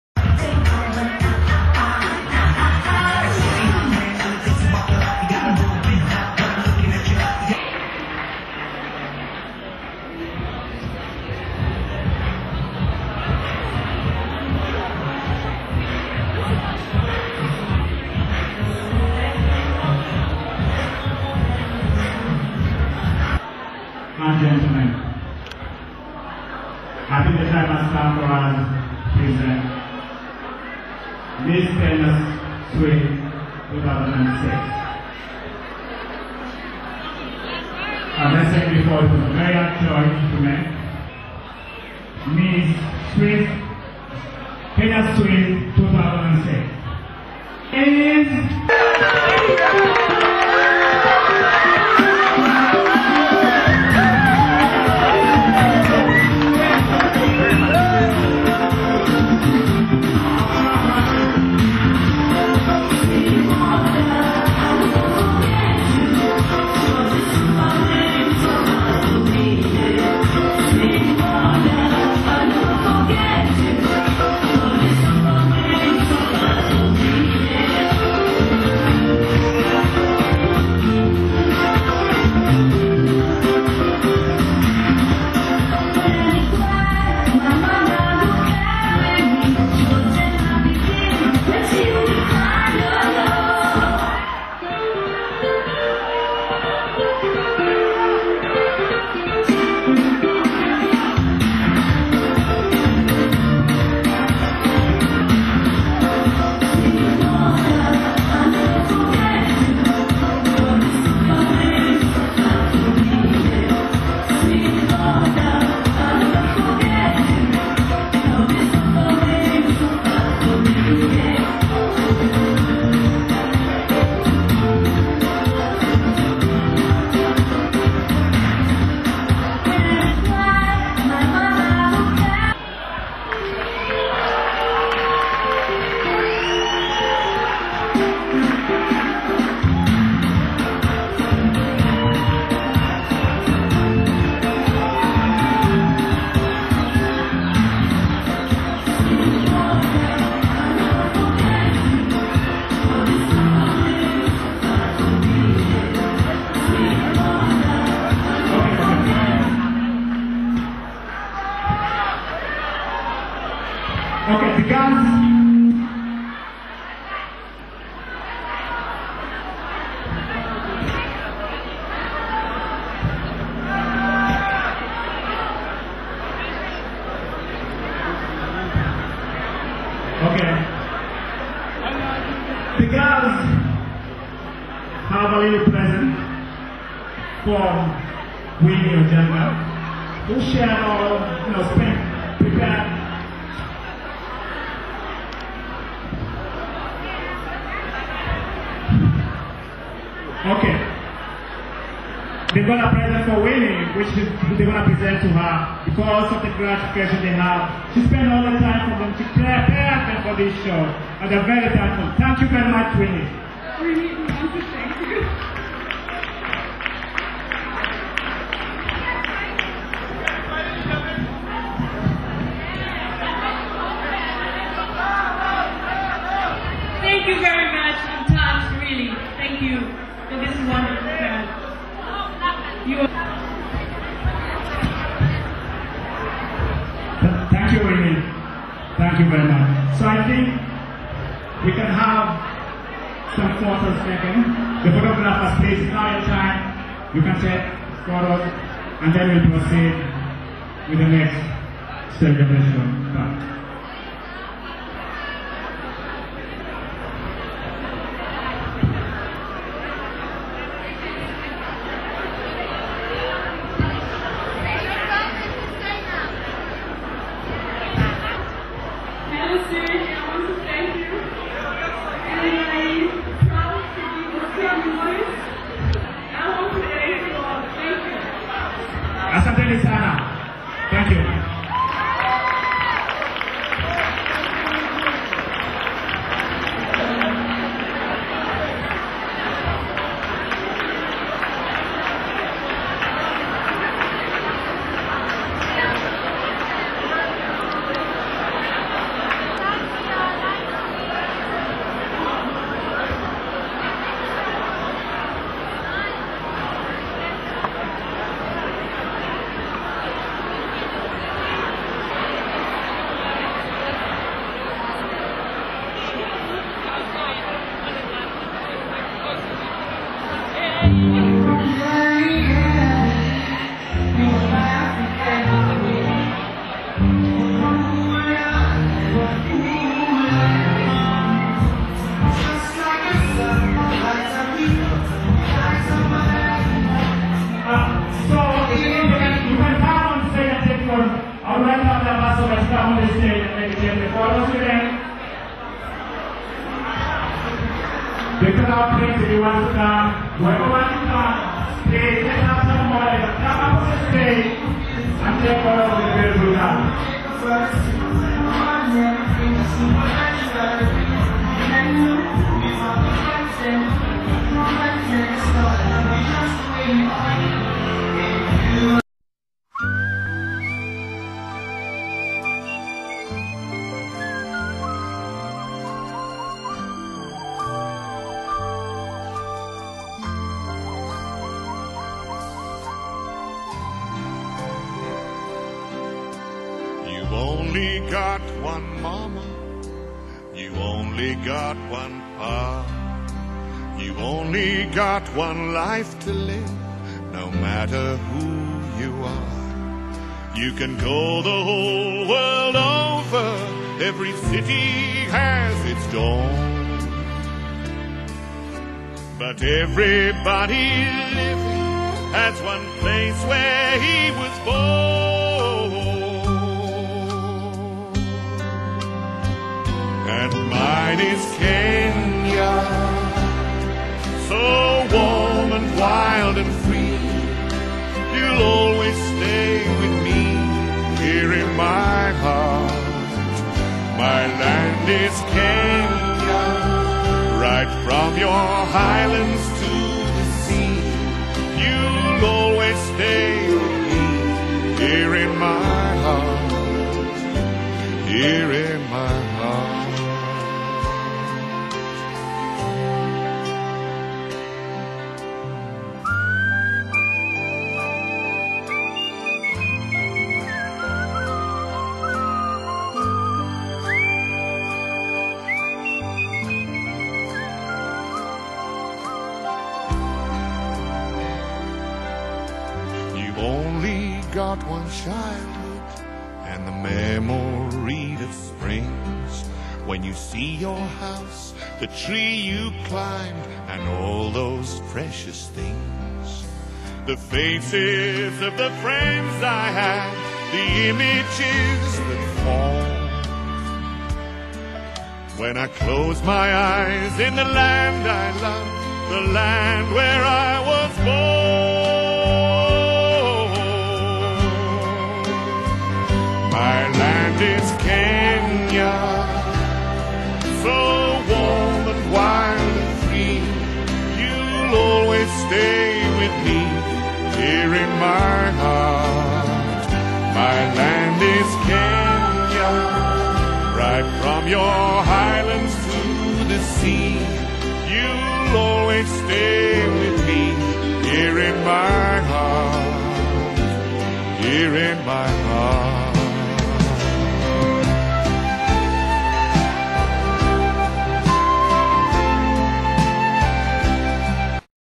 Miss Kenya Swiss 2006 & Kenya Swiss Society Gala Dinner 2006
Miss Kenya Swiss 2006 Crowning & Kenya Swiss Society Gala Dinner 2006 Guests.